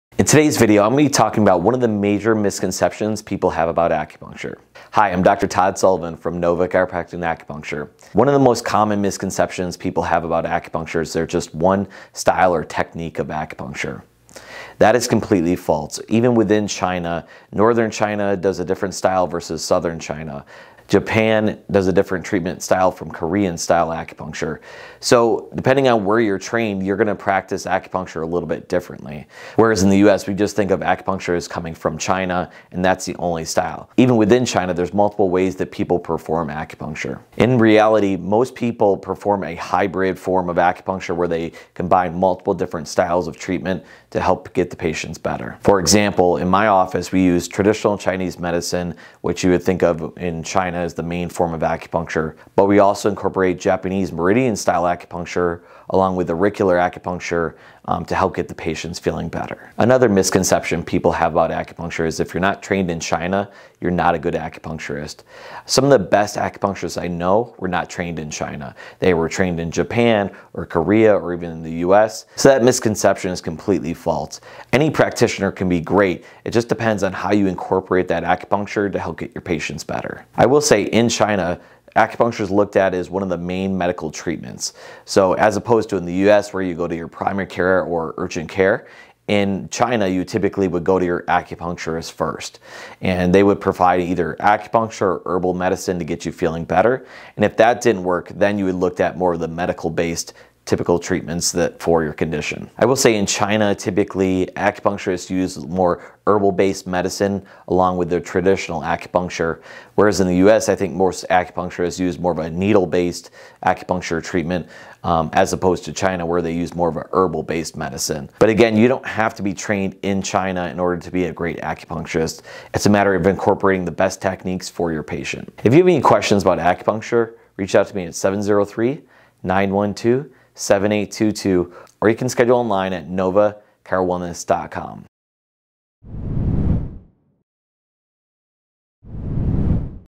Biggest Misconception About Acupuncture
In today's video discuss the most common misconceptions about acupuncture.